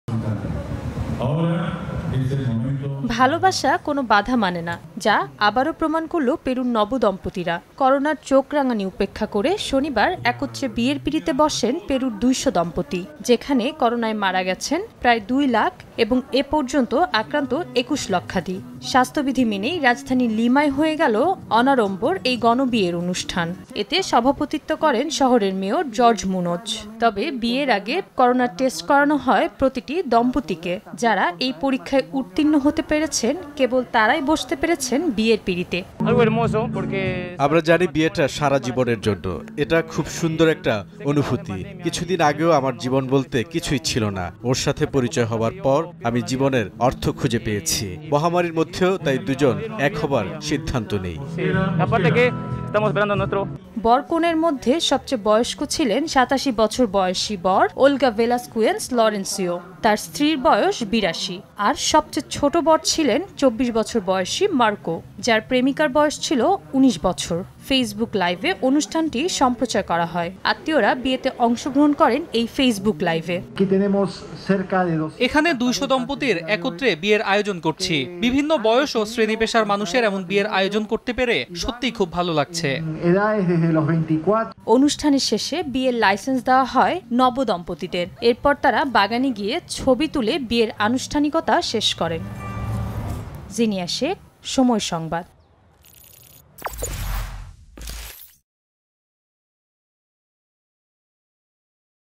করোনায় মাস্ক পরে অনুষ্ঠিত হলো গণবিয়ের অনুষ্ঠান! | Peru News | Somoy TV
করোনাও আটকাতে পারেনি তাদের বিয়ে। মুখে মাস্ক পরে বিয়ের পিঁড়িতে বসেছে ২০০ দম্পতি। গণবিয়ের এই অনুষ্ঠানটি হয়ে গেলো পেরুর লিমা শহরে। এই দুইশ' দম্পতির মাঝে ৮৭ বছর বয়সী বরও ছিলেন। বিয়ের অনুষ্ঠানে বর-কনের আত্মীয়দের ফেসবুক লাইভে অংশগ্রহণের সুযোগ করে দেয়া হয়।

Corona could not stop their marriage. 200 couples are sitting on the wedding pedestal after wearing face masks. The wedding took place in Lima, Peru. Among the 200 couples was a 7-year-old groom. The relatives of the bride and groom were given the opportunity to participate in the Facebook Live.